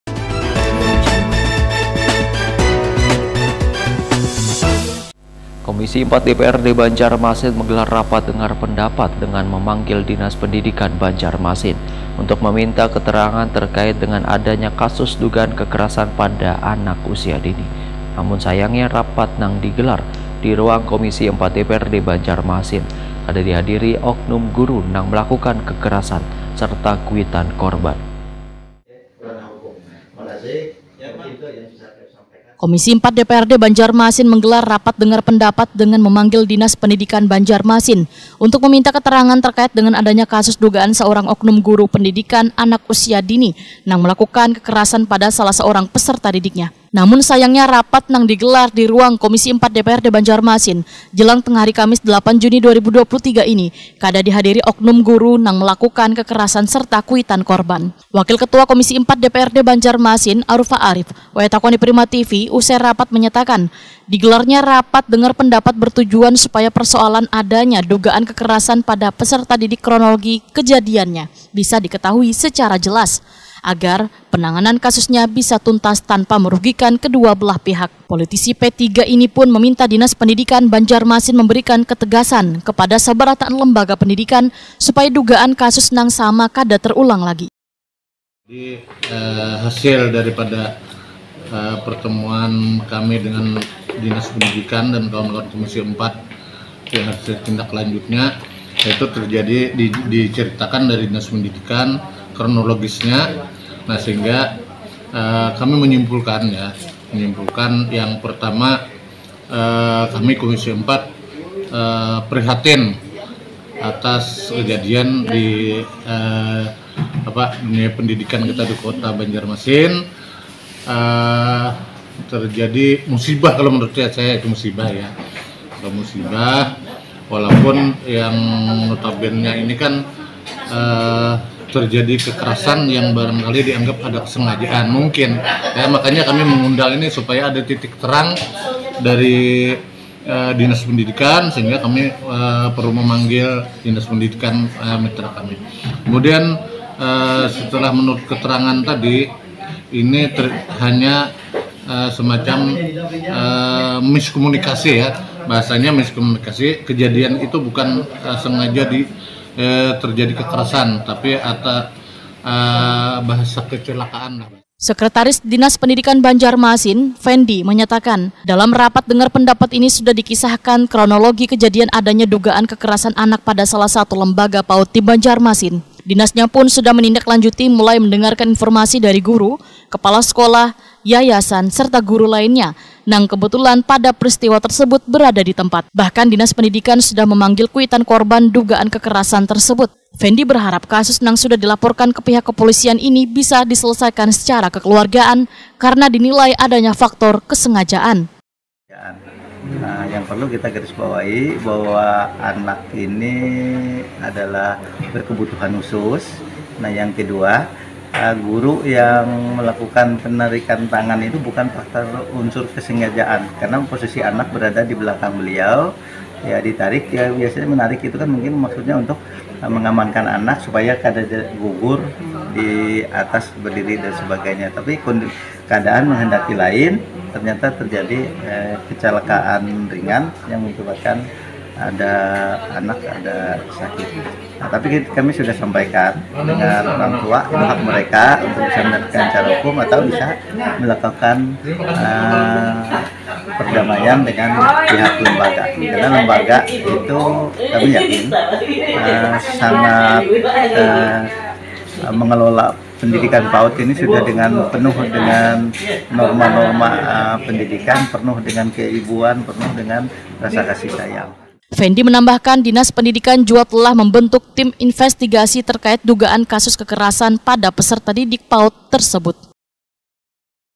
DINAS PENDIDIKAN BANJARMASIN TELAH MEMBENTUK TIM INVESTIGASI TERKAIT DUGAAN KASUS KEKERASAN
Komisi IV DPRD Banjarmasin menggelar rapat dengar pendapat dengan memanggil Dinas Pendidikan Banjarmasin/  untuk  meminta keterangan terkait dengan adanya kasus dugaan saurang oknum guru Pendidikan Anak Usia Dini nang melakukan kekerasan pada salah saurang peserta didiknya//